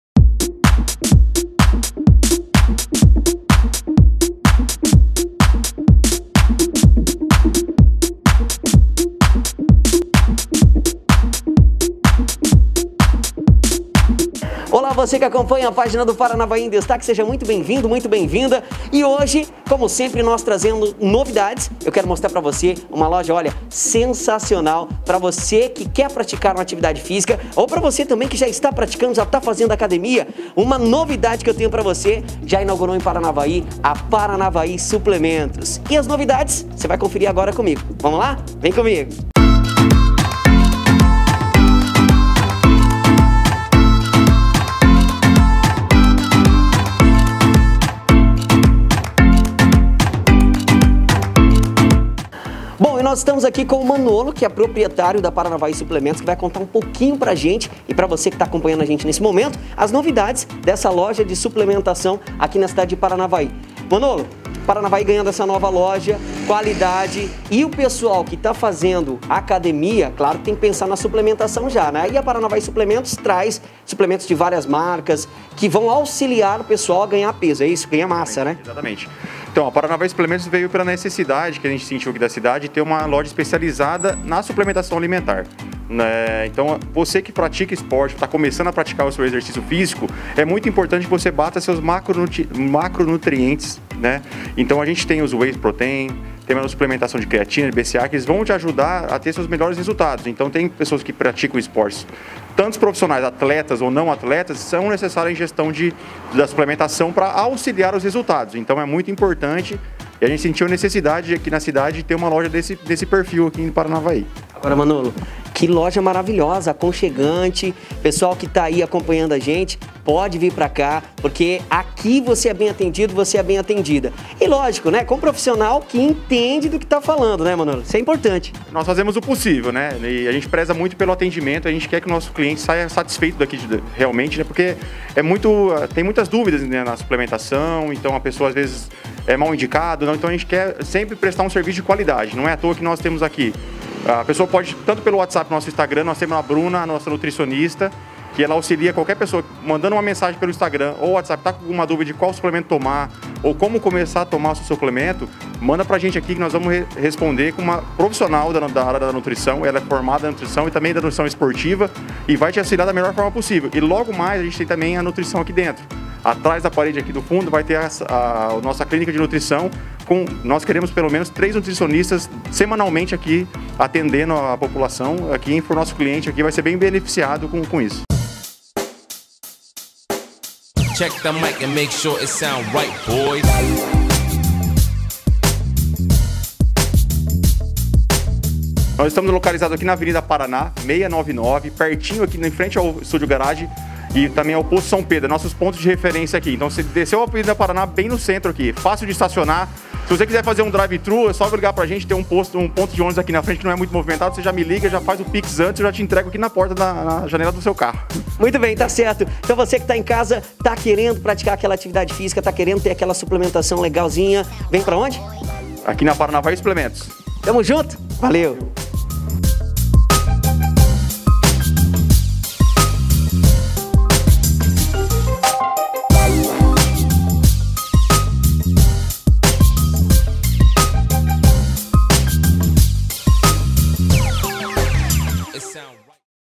Conheça a Paranavaí Suplementos
Já inaugurou em Paranavaí a maior loja de suplementos de Paranavaí e Região a Paranavaí Suplementos. Bora conhecer?